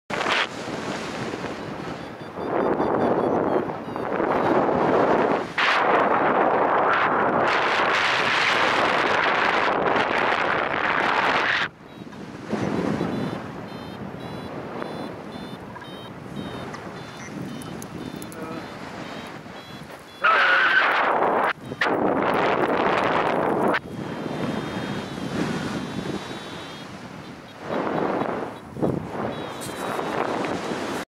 DragonView.ch Tandem Paragliding Adventure Switzerland - 4K Unedited
Flying over Kleiner Mythen. The most amazing Swiss Alps flying location closest to Zurich, Switzerland. We fly all over the Swiss Alps!